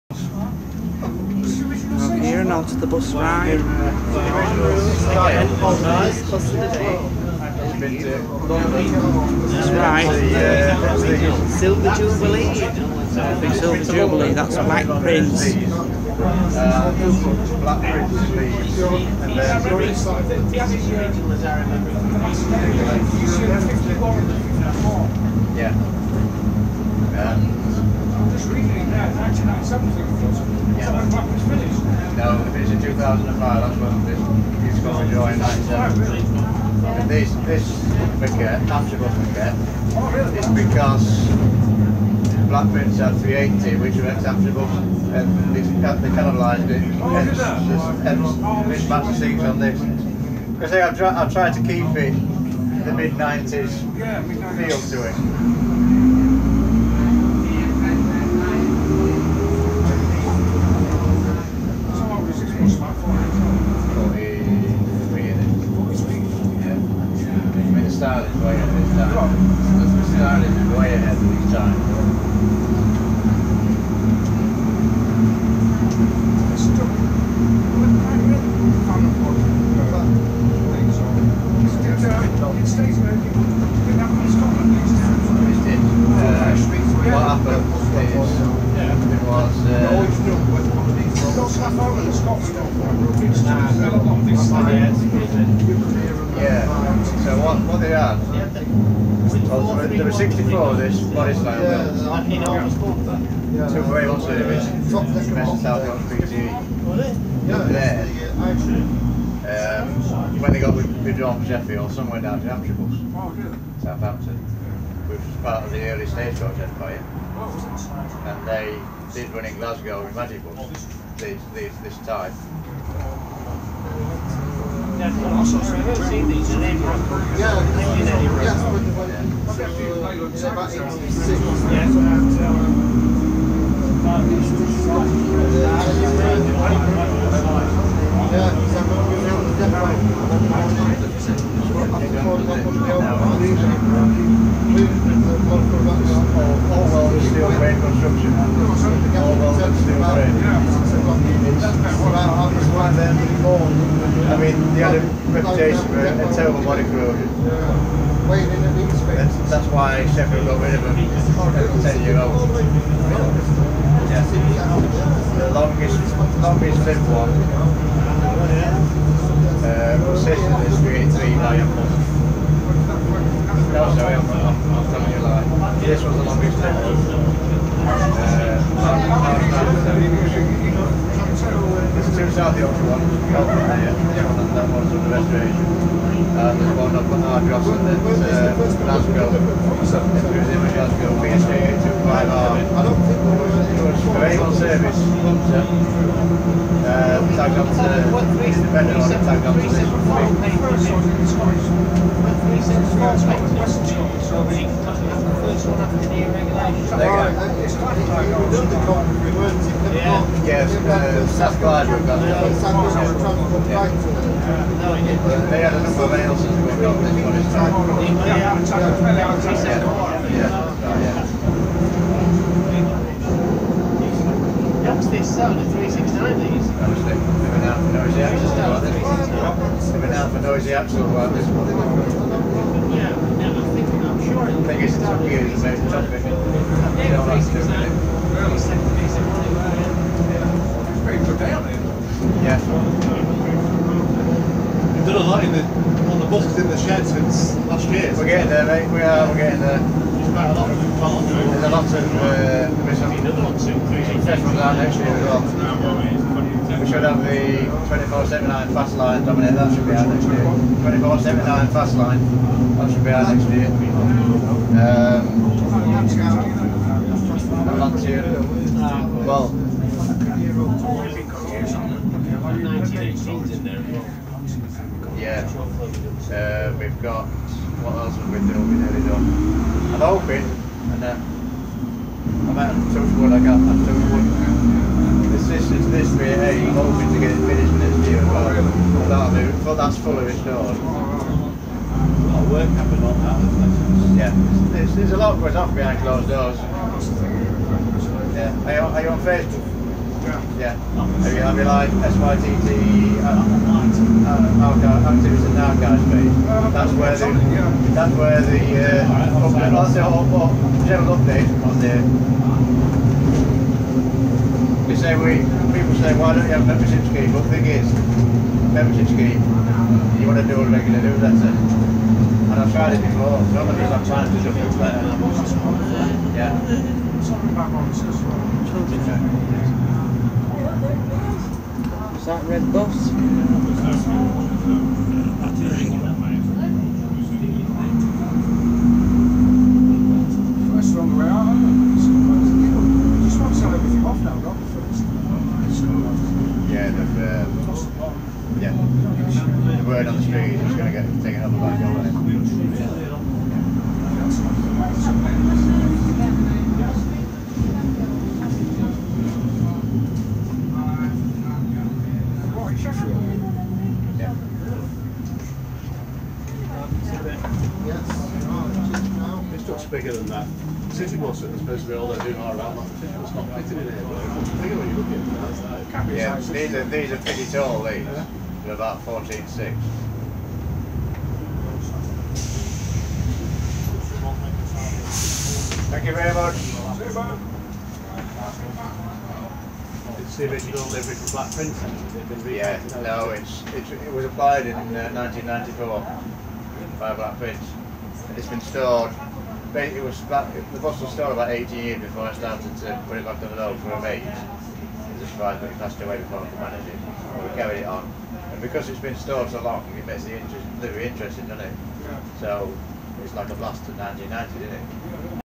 Black Prince Volvo Ailsa 377 (LWB 377P) part 1
This is an ex South Yorkshire Transport, Black Prince Volvo Ailsa double decker bus that's in a vanhool mcardle body.